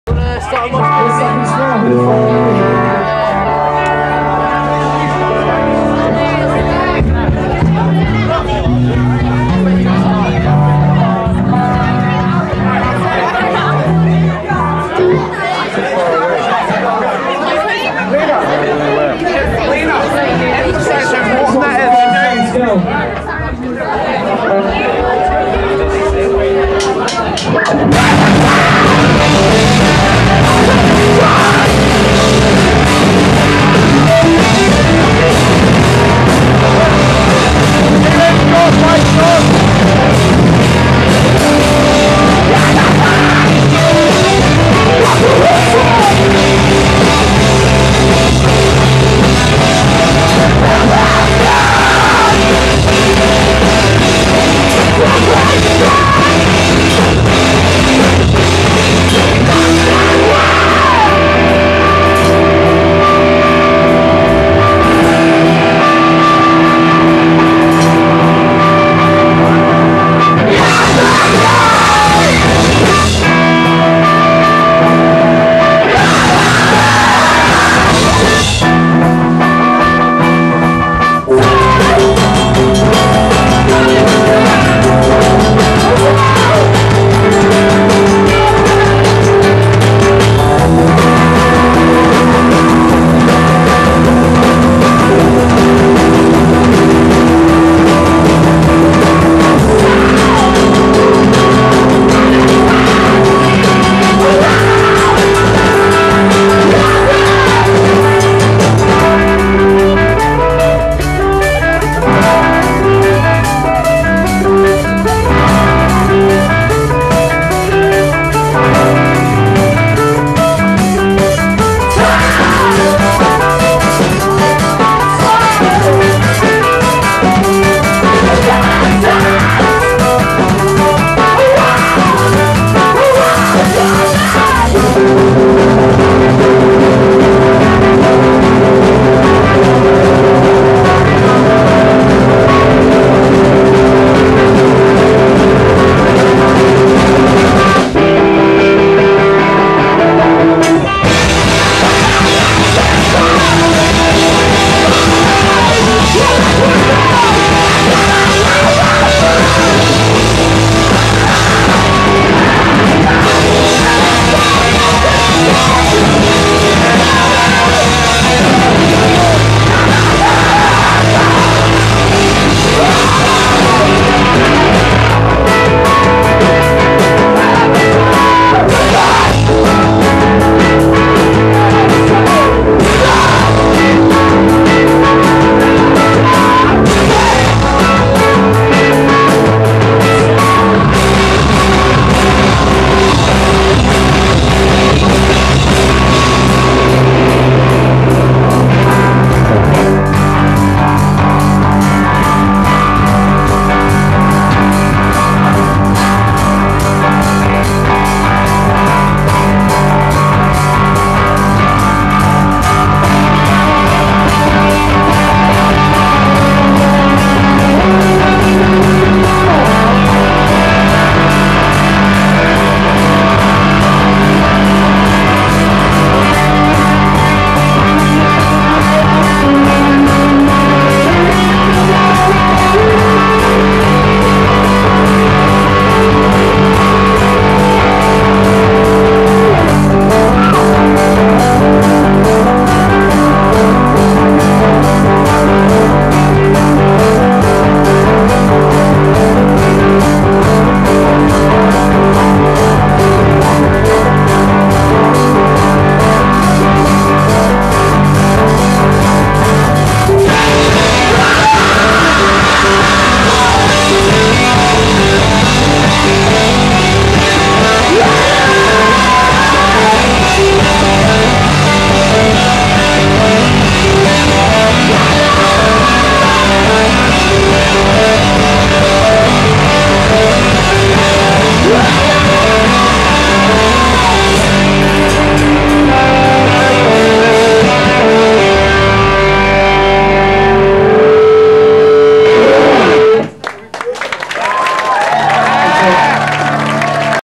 Oldschool Secondsmile Live (Part 2)
Playing live at Bridport Royal Oak (Now named The Market House) 2004. Ross Smithwick and Thomas Warne on Guitar, Philip Wrixton on Bass, Olly Goodland on Vocals & Fiann Smithwick on Drums.

(Re-uploaded because I found the bigger versions and Youtube upload limits have changed since 2009)

Part 2